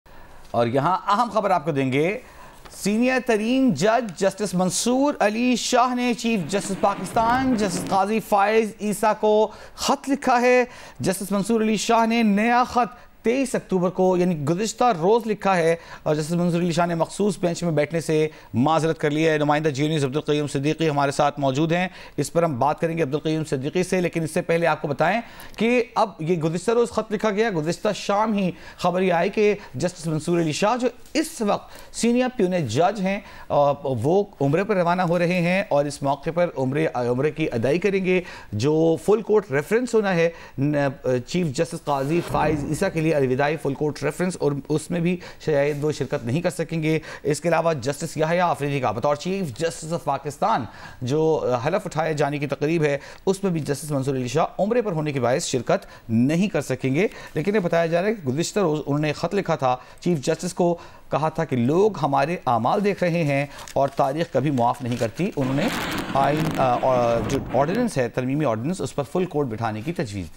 History Never Forgive - Justice Masoor Ali Shah Bold Letter to CJP Qazi Faez Isa -Breaking News
In a powerful second letter to the Chief Justice, Justice Mansoor emphasizes that "history never forgives." This statement raises significant questions about judicial accountability and the future of Pakistan's legal system. Tune in for an in-depth analysis of the letter's implications and reactions from the legal community!